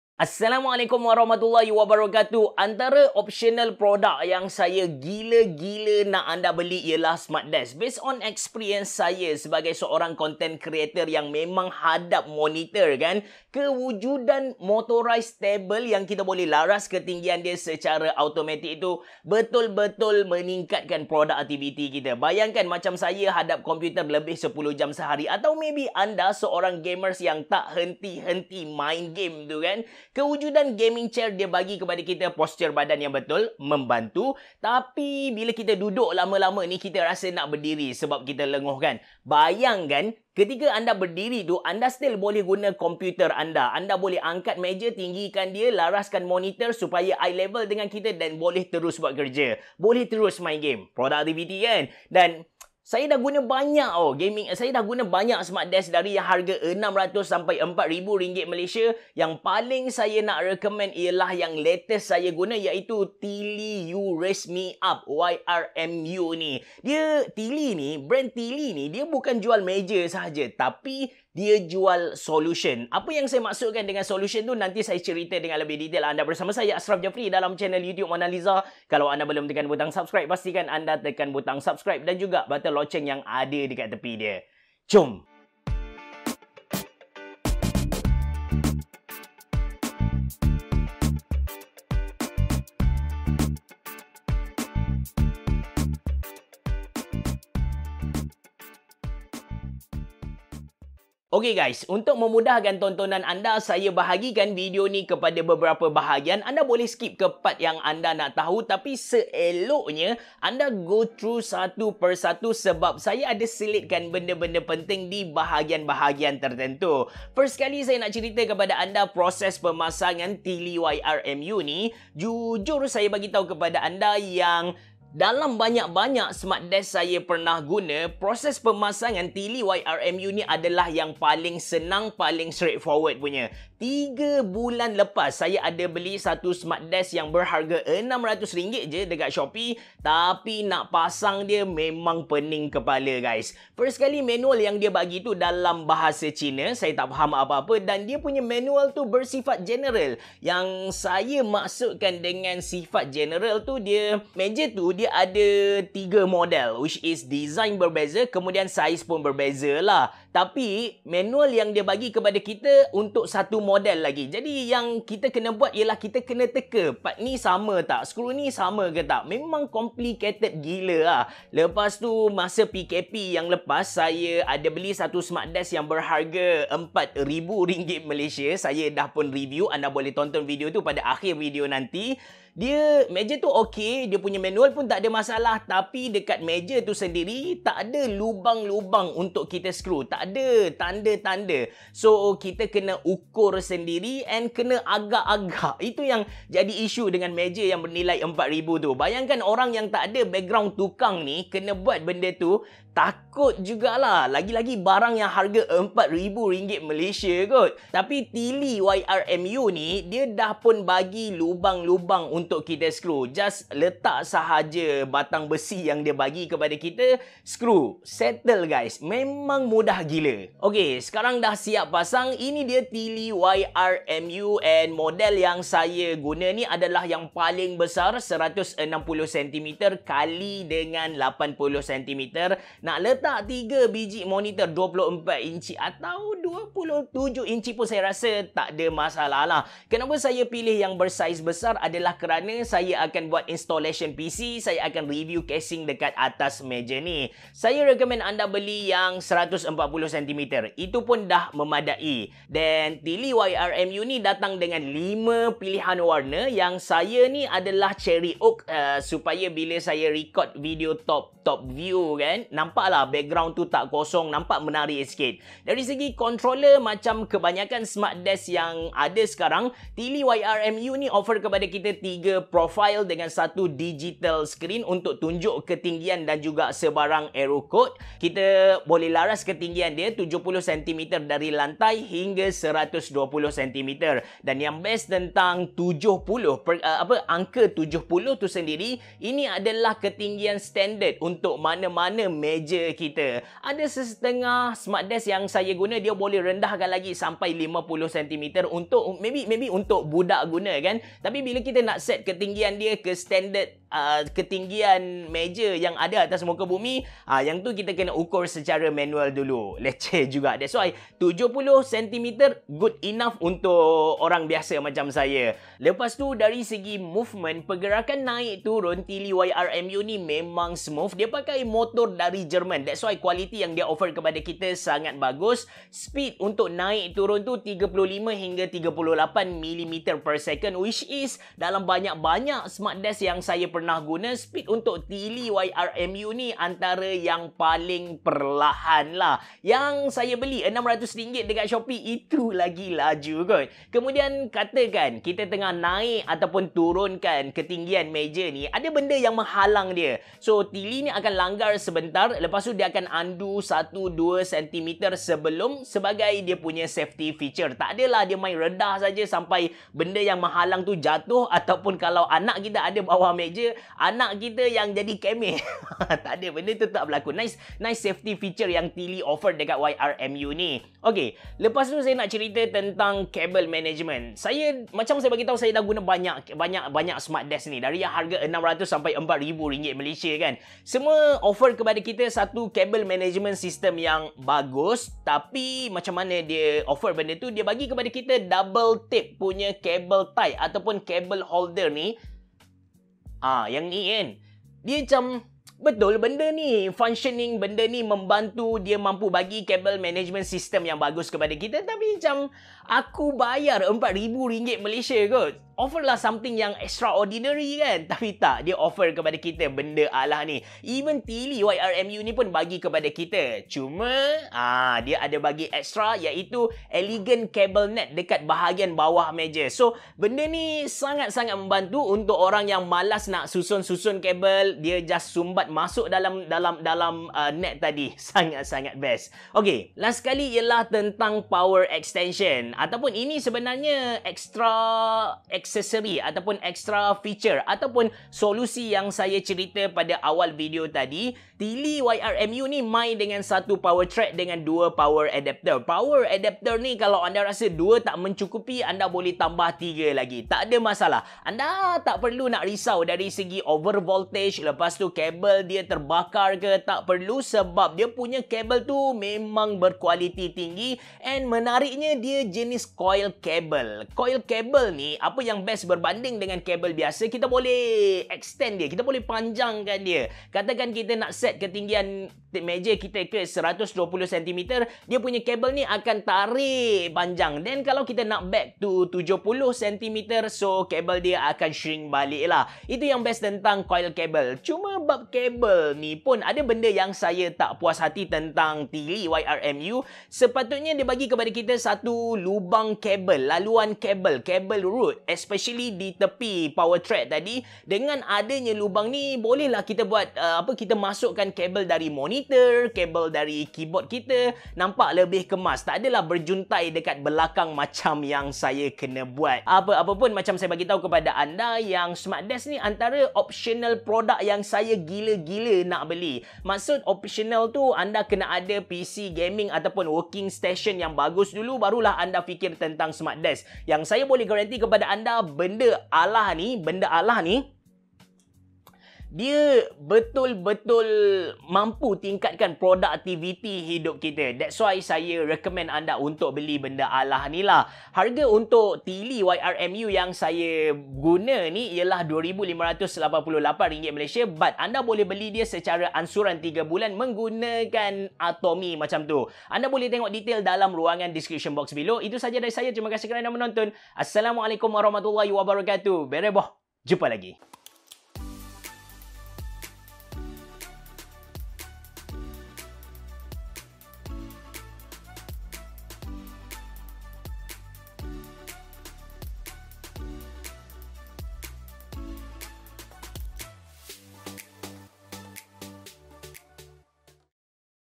Meja Komputer Paling BERBALOI BELI,Kalah Meja RM4k! Review Tyyli YRMU Smart Desk
Ini adalah Smart Desk (Motorized Table) yang paling BERBALOI saya pernah guna. Company ini bukan jual meja tapi mereka jual SOLUTION. Beli di sini